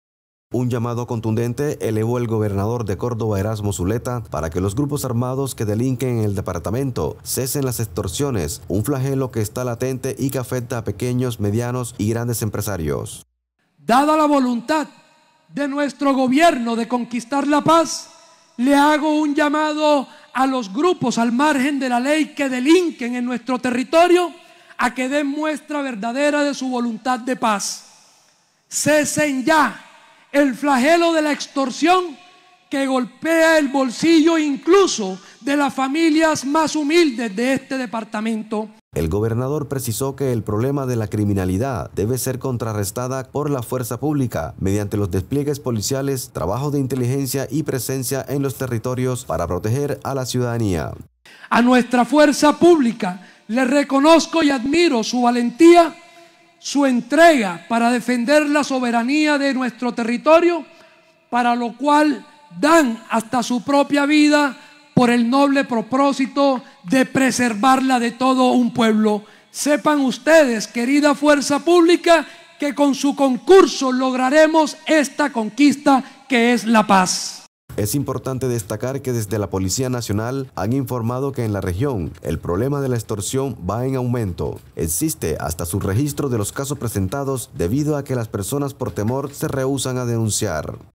Erasmo Zuleta, gobernador de Córdoba, pide a grupos armados que cesen las extorsiones
Córdoba | El mandatario regional sostuvo que este flagelo no solo impacta a los empresarios y ganaderos, sino a las familias más humildes de la región.